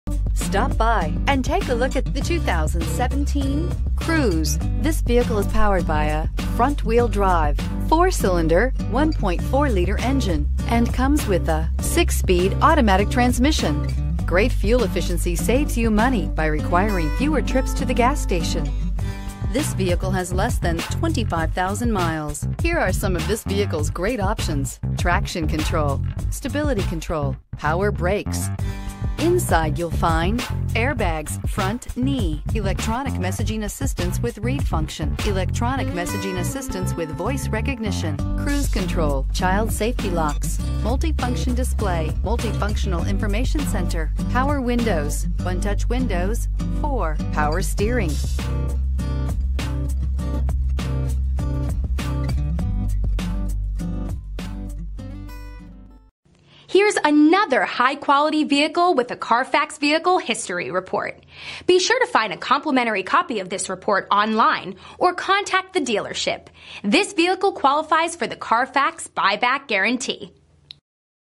2017 Chevrolet Cruze Springfield Township NJ 18599TDZ
2017 Chevrolet Cruze 4dr Sdn 1.4L LT w/1SD

Springfield Acura
243 US-22

This 2017 Chevrolet Cruze 4dr Sdn 1.4L LT w/1SD is complete with top-features such as the stability control, traction control, anti-lock brakes, dual airbags, side air bag system, digital display, and airbag deactivation.  This one's a deal at $16,777.  With a 4-star crash test rating, this is one of the safest vehicles you can buy.  For a good-looking vehicle from the inside out, this car features a gorgeous pepperdust metallic exterior along with a jet black interior.  It has the best mileage with 30 MPG in the city and 40 MPG on the highway.